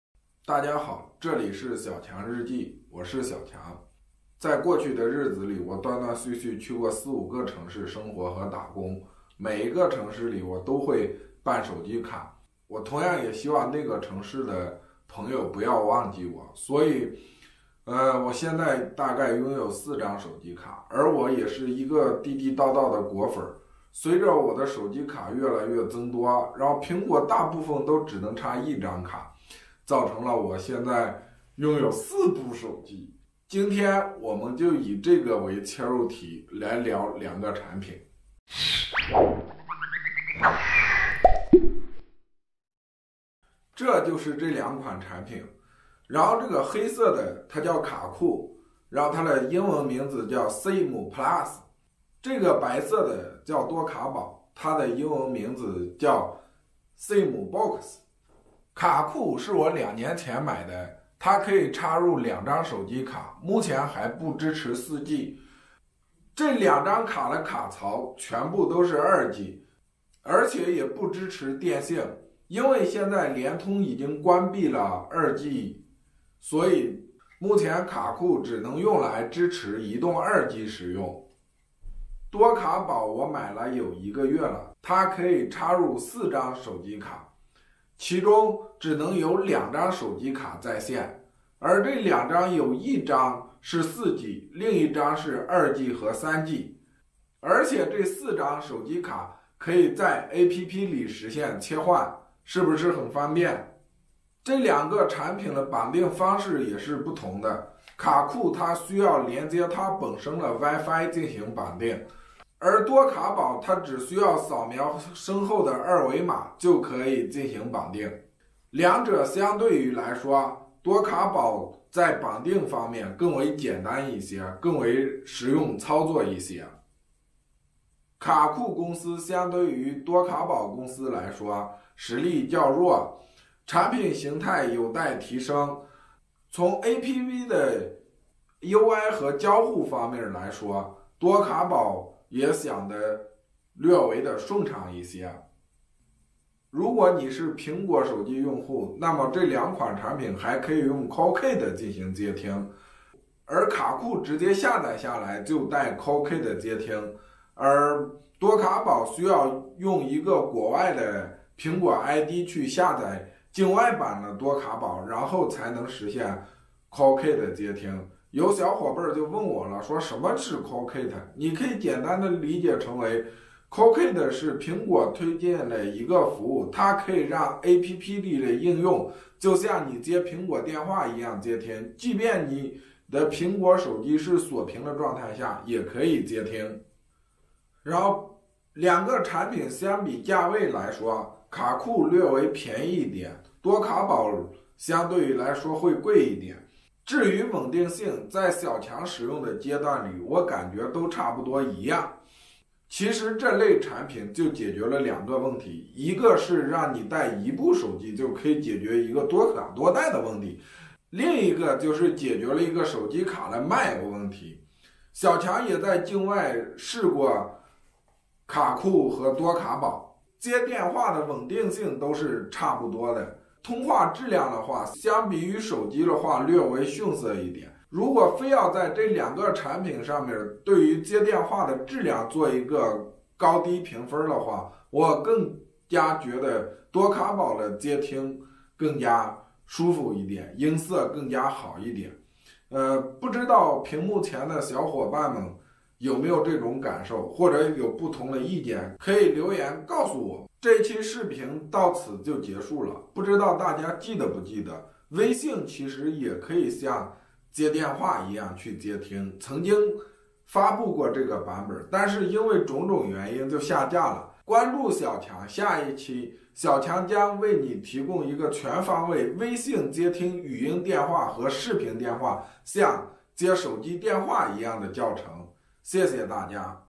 【小强日记】出国神器-免国际漫游费-多卡宝simbox和卡酷simplus开箱评测-Sim卡国际版漫游-体验手机多卡多待-支持全网手机卡，告别带多部手机出门局面，iPhone实现双卡双待免费国际漫游
sim手机卡多的朋友，出门就得带多部手机，经过很多平台推荐，都让购买多卡宝simbox和卡酷simplus，多卡宝simbox支持全网通，支持四张手机卡插入，支持2g，3g，4g，两张手机卡可以在线，卡酷simplus开箱时间最长，评测最久，支持2g，不支持3g和4g。购买的朋友需要注意了，购买以后，从此告别带多不手机的局面。再也不需要带多部手机，iPhone实现双卡双待，就可以实现多卡多待，免费国际漫游哦。需要购买的小伙伴可以加我的Telegram可以便宜30-50元不等。
自媒体平台搜索“小强日记iXiaoQiang”认准小强哦！
官方QQ群：23752666